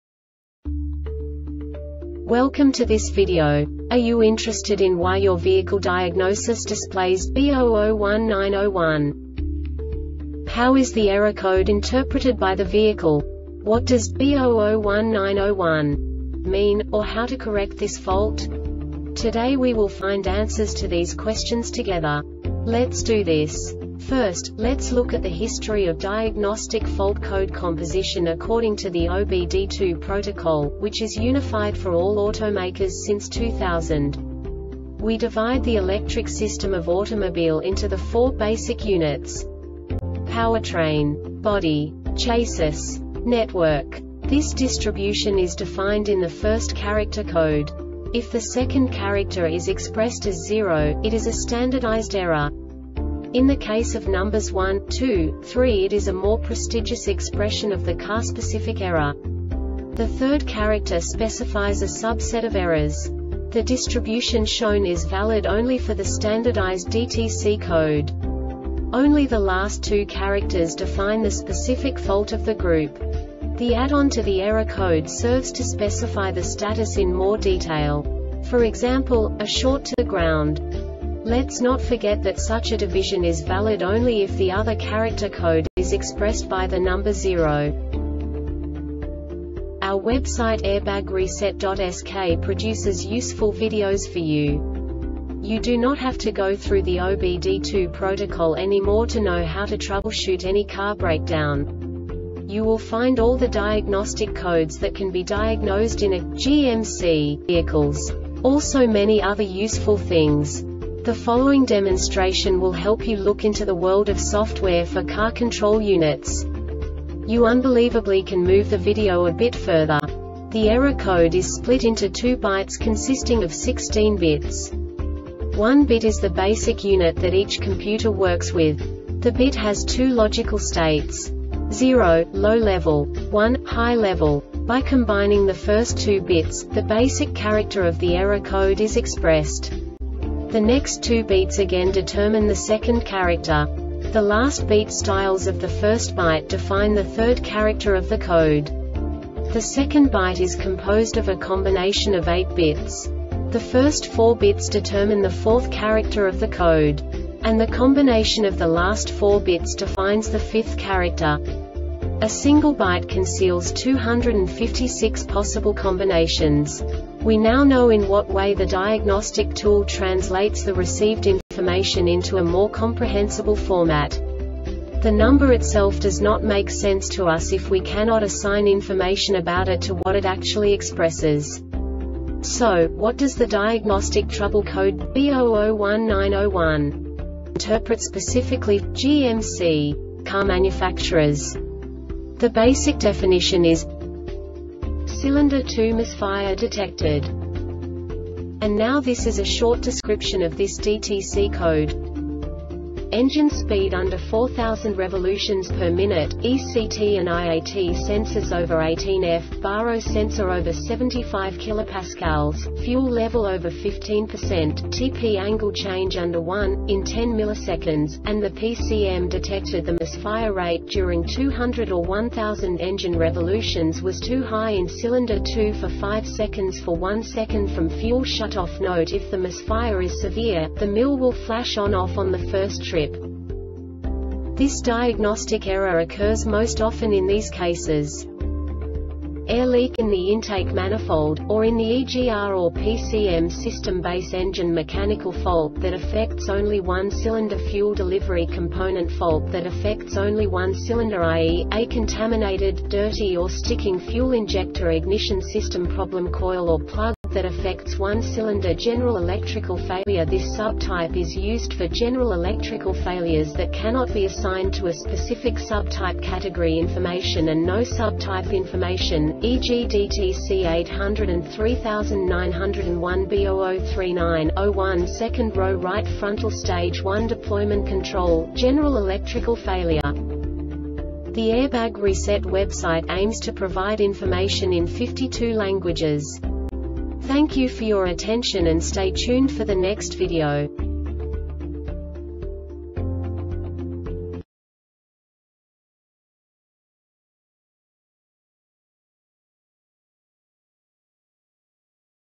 DTC GMC B0019-01 second Short Explanation
Contents:
0:21 Basic DTC analysis according to OBD2 protocol standard.
1:48 Insight into programming
2:58 A diagnostically understandable description of the Diagnostic Trouble Code
3:05 Basic error definition

Make:
GMC
Code:
B0019 01
Definition:
Cylinder 2 Misfire Detected
Description:
Engine speed under 4000 rpm,ECT and IAT sensors over 18ºF,BARO sensor over 75 kPa,fuel level over 15%,TP angle change under 1º in 10 ms,and the PCM detected the misfire rate during 200 or 1000 engine revolutions was too high in Cylinder 2 for 5 seconds for 1 second from fuel shutoff.Note: If the misfire is severe,the MIL will flash on or off on the 1st trip!
Cause:
Air leak in the intake manifold,or in the EGR or PCM systemBase engine mechanical fault that affects only one cylinderFuel delivery component fault that affects only one cylinder (i. e. ,a contaminated,dirty or sticking fuel injector)Ignition system problem (coil or plug) that affects one cylinder
Failure Type: